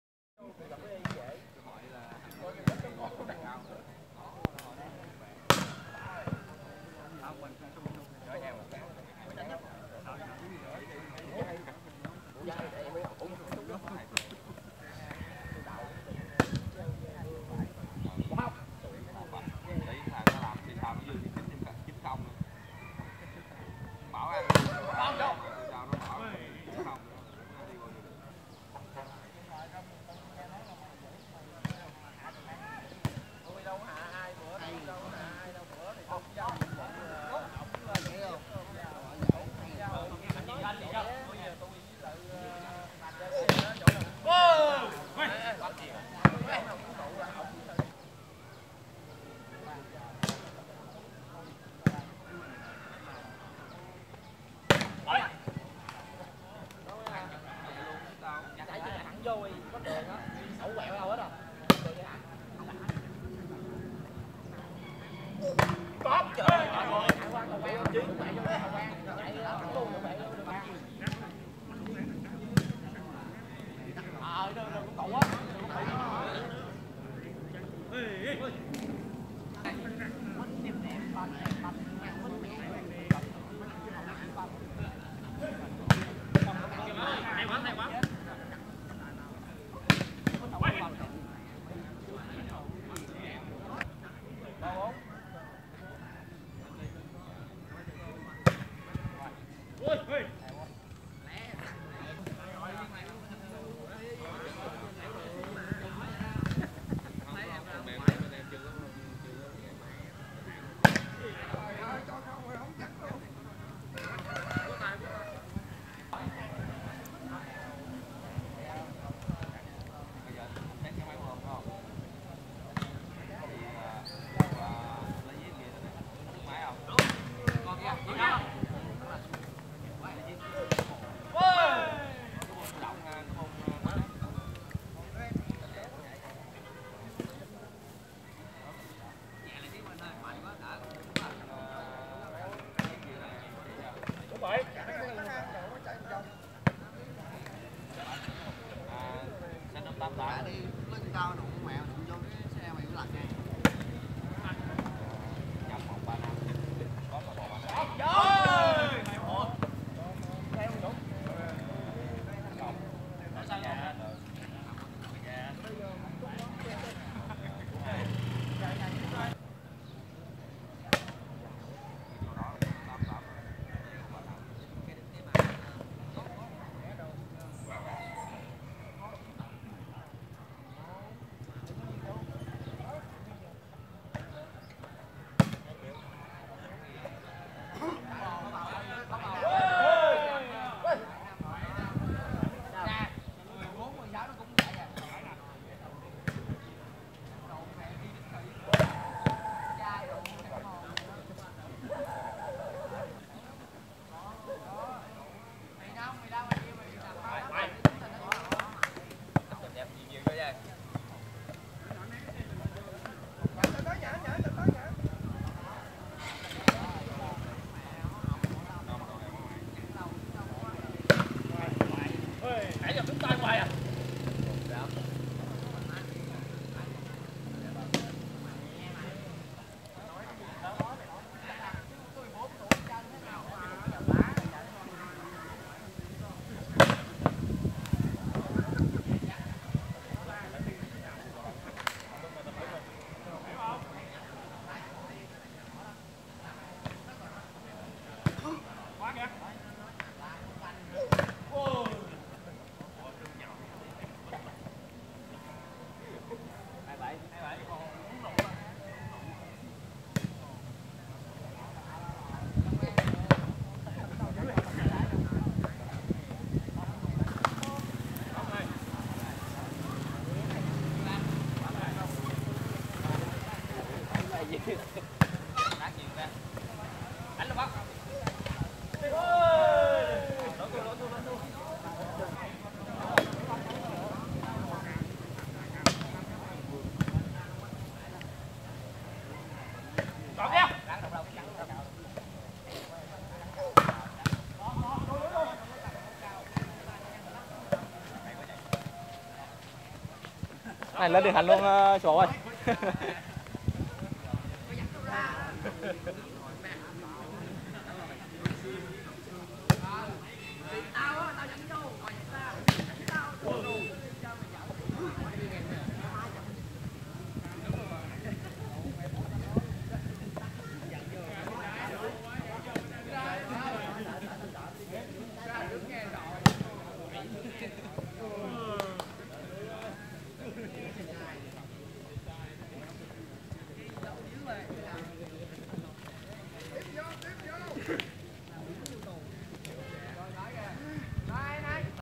Giải bóng chuyền 4-4 Tha La mở rộng 2017 (An Hòa - Trảng Bàng - Tây Ninh)
ĐH Ngân Hàng vs ĐH Bách Khoa (sec cuối)